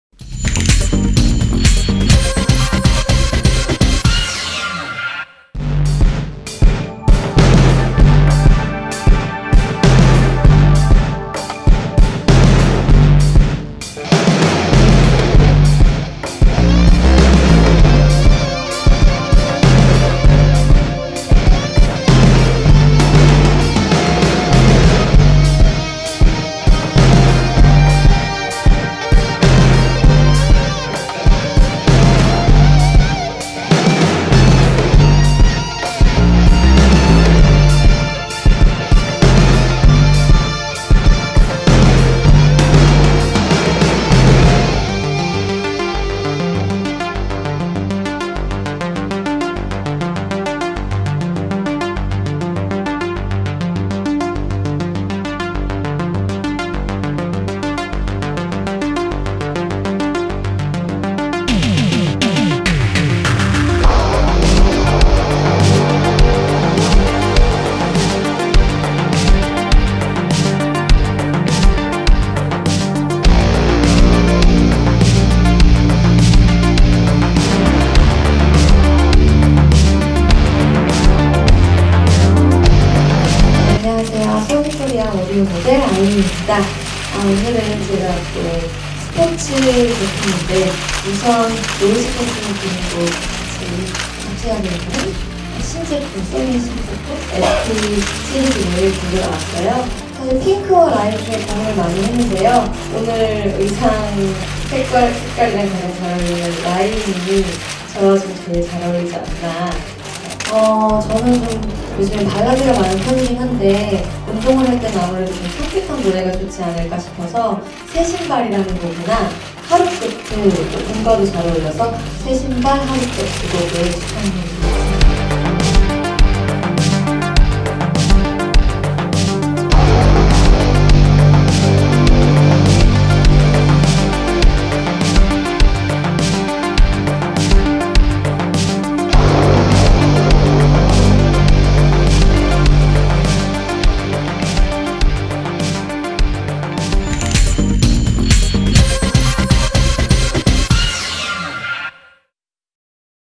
[S영상] 아이유(IU), '누가 뭐래도 예쁜 이지은' (소니)
[스타데일리뉴스=소가영 기자] 아이유가 23일 오전, 서울 중구 웨스틴조선호텔서울에서 열린 소니 스포츠용 무선 노이즈 캔슬링 이어폰 SP 시리즈 출시 행사에 참석했다.

소니가 새롭게 선보인 무선 이어폰 SP 시리즈 2종은 세계 최초 방수 노이즈 캔슬링 완전 무선 이어폰과 넥밴드 이어폰으로 가벼운 비나 땀이 닿아도 문제 없는 방수 규격을 갖췄다.